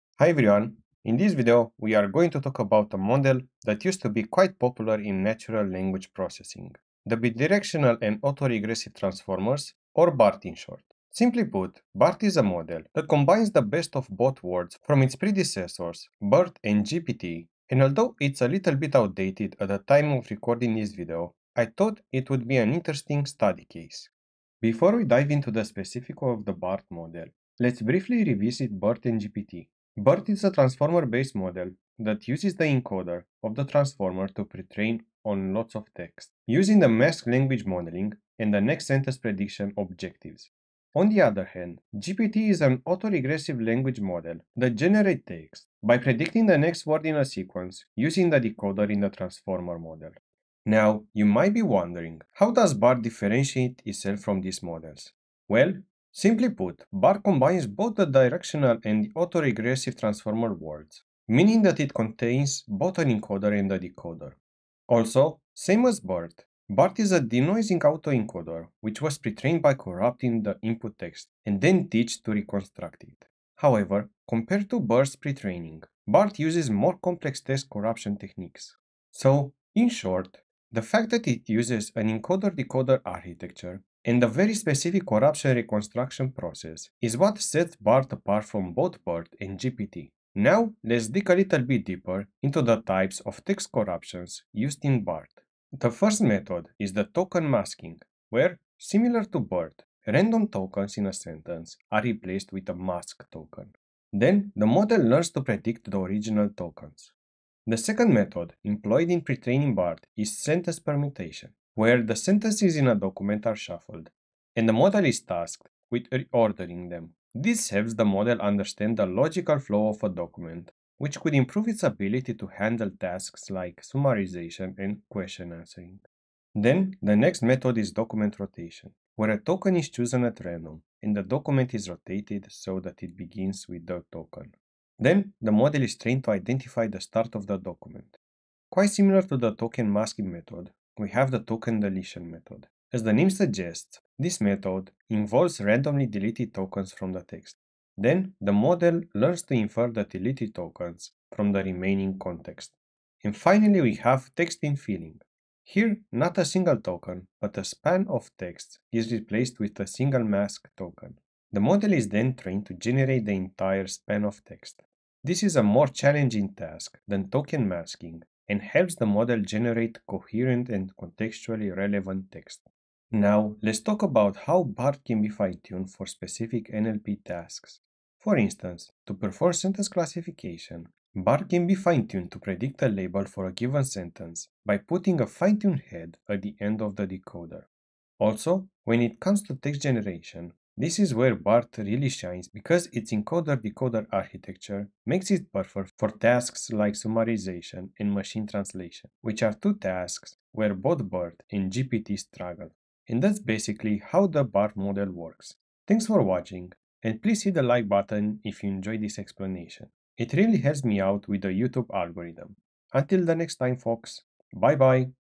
BART Explained: Denoising Sequence-to-Sequence Pre-training
In this video we talk about how the Bidirectional and Auto-Regressive Transformer (BART) model works. Compared to other models like BERT or GPT, BART uses an encoder-decoder architecture that was trained using a combination of denoising autoencoder objectives and autoregressive language modeling.

*References*
"BART: Denoising Sequence-to-Sequence Pre-training for Natural Language Generation, Translation, and Comprehension" paper

*Contents*
00:00 - Intro
00:24 - BPE Encoding
00:50 - BERT vs GPT vs BART architectures
01:35 - BART text corruptions
02:52 - BART Fine-tuning
03:22 - Outro

► Bitcoin (BTC): 3C6Pkzyb5CjAUYrJxmpCaaNPVRgRVxxyTq
► Ethereum (ETH): 0x9Ac4eB94386C3e02b96599C05B7a8C71773c9281
► Cardano (ADA): addr1v95rfxlslfzkvd8sr3exkh7st4qmgj4ywf5zcaxgqgdyunsj5juw5
► Tether (USDT): 0xeC261d9b2EE4B6997a6a424067af165BAA4afE1a